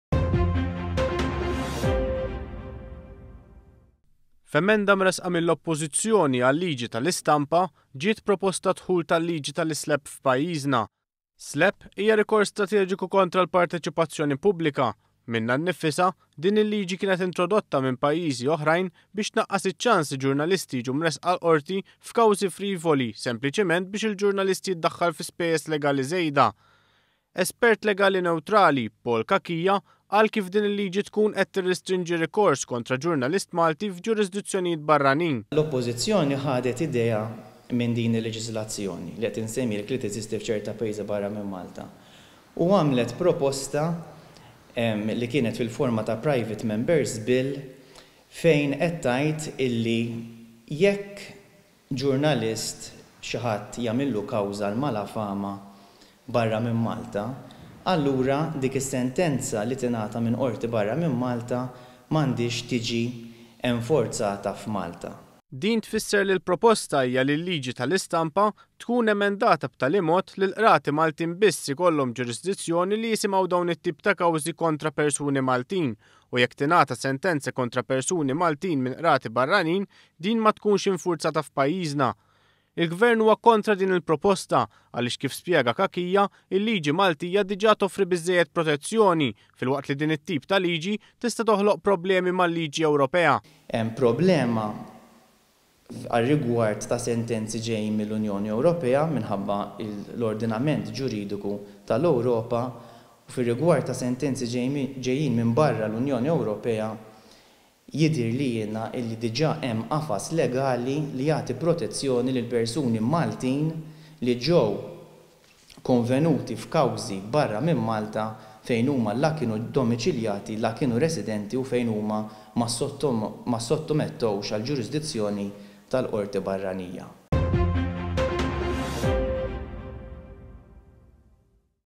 L-ISLAPP F’EMENDA FRIVOLA U PROBLEMATIKA MAL-LIĠI EWROPEJA
ONE.COM.MT - L-oppożizzjoni pproponiet li issir emenda fil-liġi l-ġdida tal-istampa li tinkludi l-iSlapp f’pajjiżna.  Din il-proposta tnaqqas mid-dritt tal-pubbliku li jitlob ġurnalist iwieġeb fuq dak li kiteb f’qorti internazzjonali.

Din l-emenda tista’ toħloq problemi mal-liġi ewropeja filwaqt li protezzjoni simili għall-ġurnalist li jitressaq f’qorti barra l-ewropa hija diġa provduta mil-liġi.